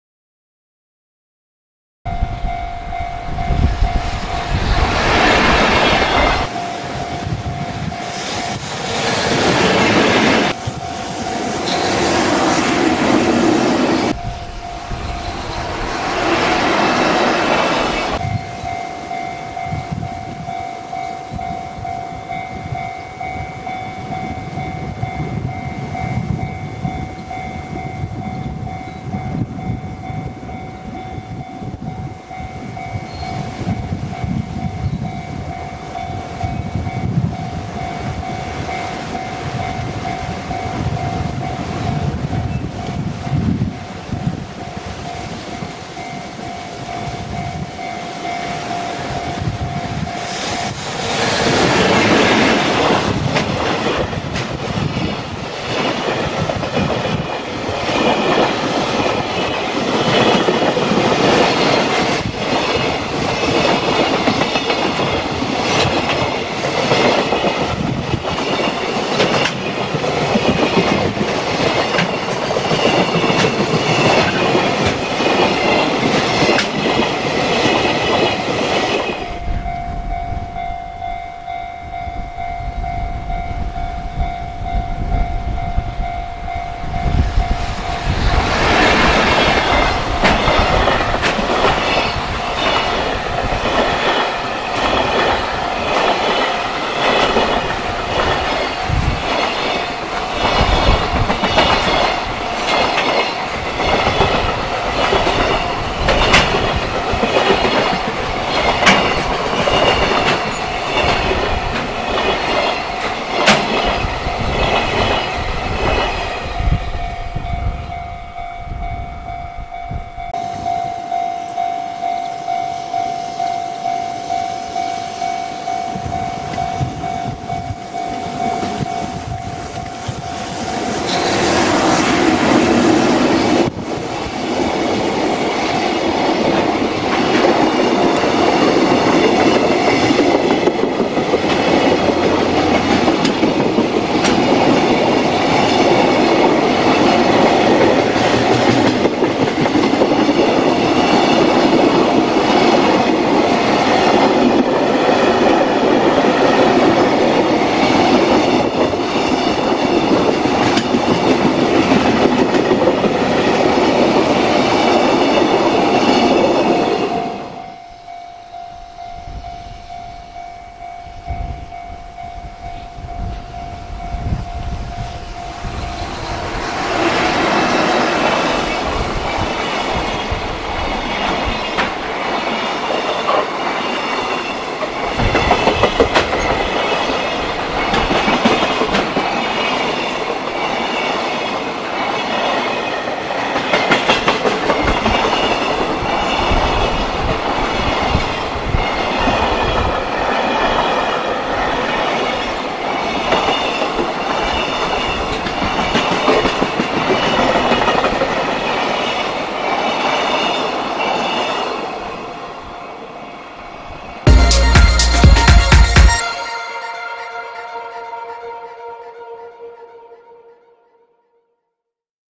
JR貨物 2024/01/16 遠州名物の空風吹き荒れる朝の貨物列車2本 東海道本線
強風吹き荒れる中でのカメラブレあります。
朝の貨物列車1060レ 1071レの2本